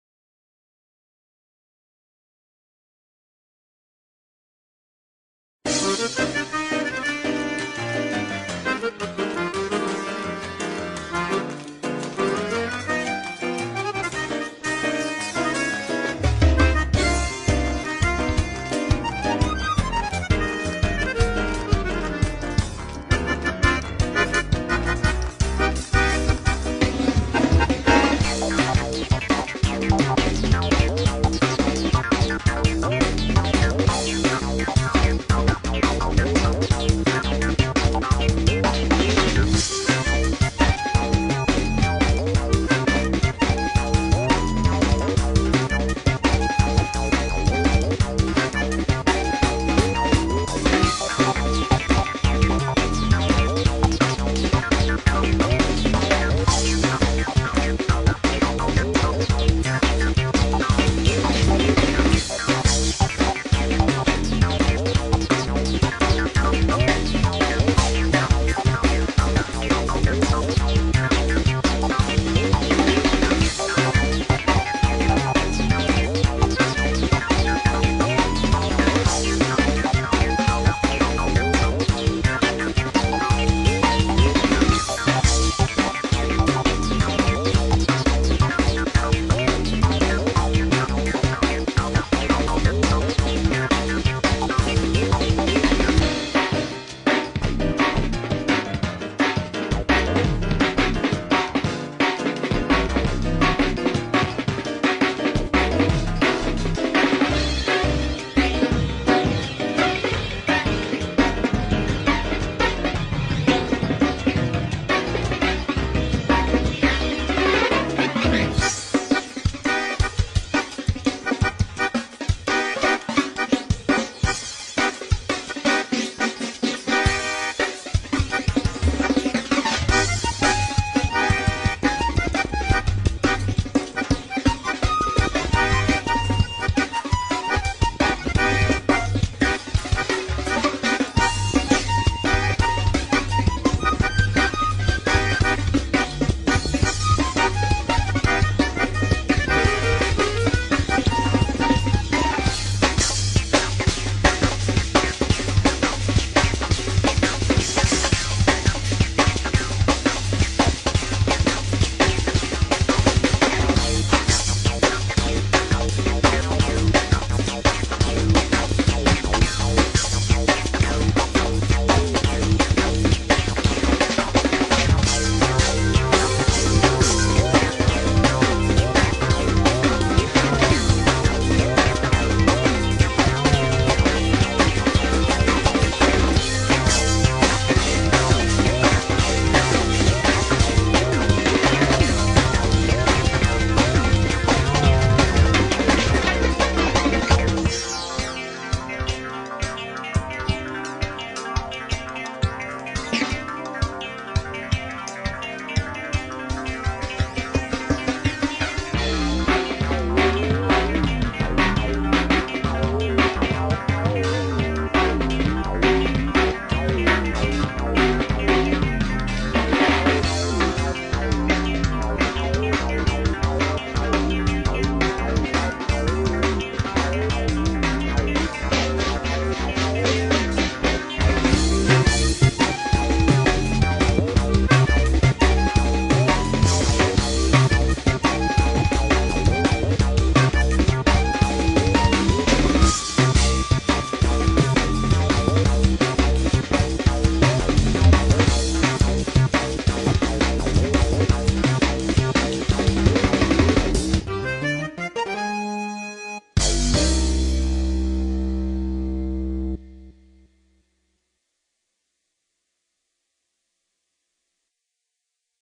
Astor Drum´n Bass
fantastic video of daniel baeder playing astor drum n bass!